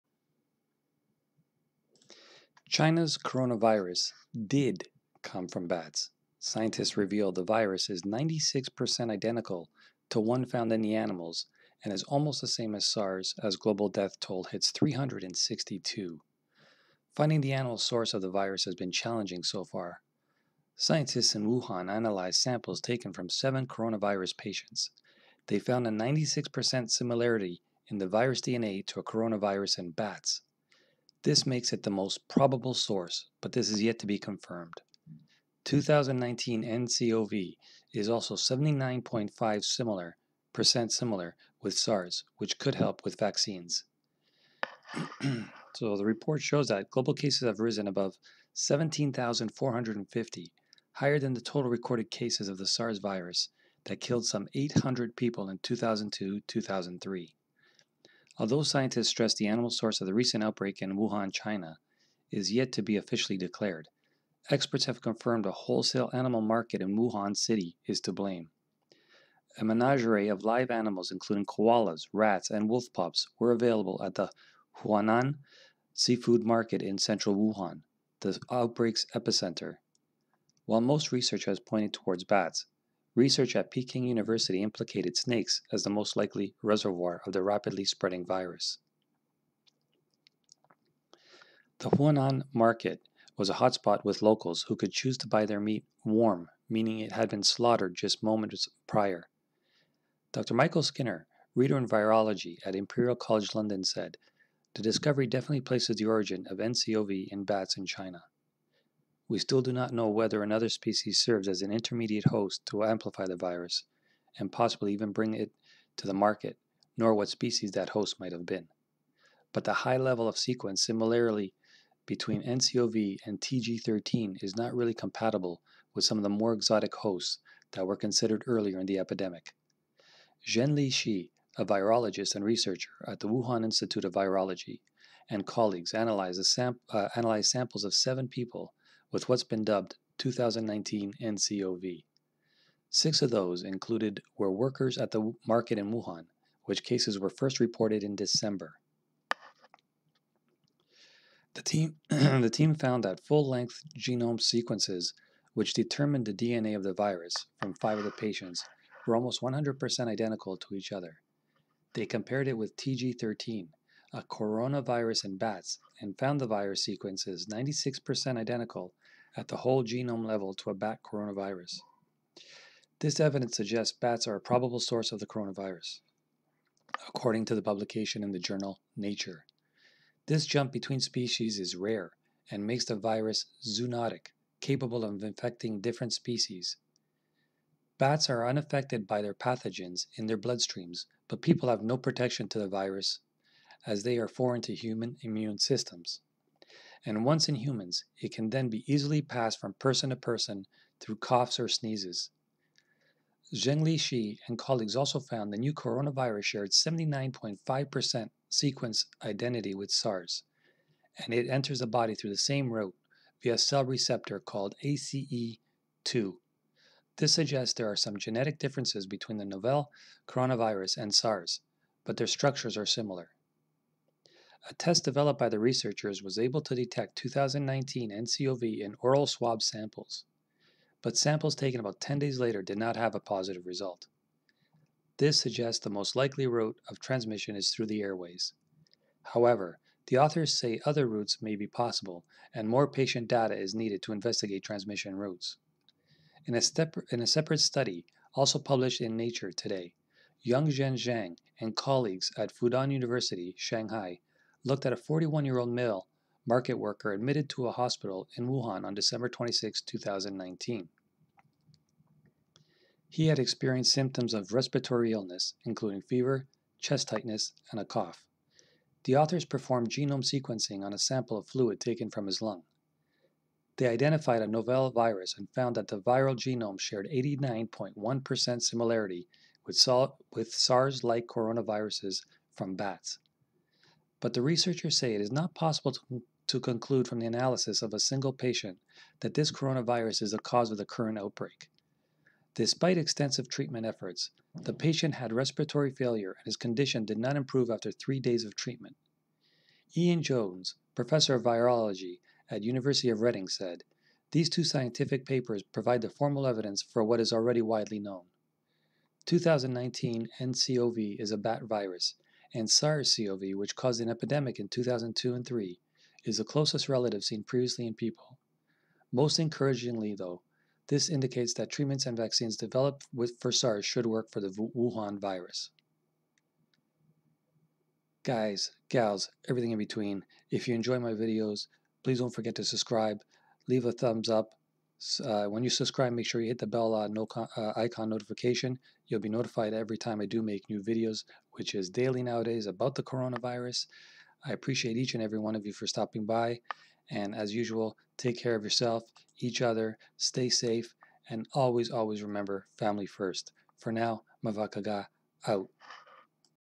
China's coronavirus DID come from bats Scientists reveal the virus is 96% identical to one found
Scientists reveal the virus is 96% identical to one found in the animals and is almost the same as SARS as global death toll hits 362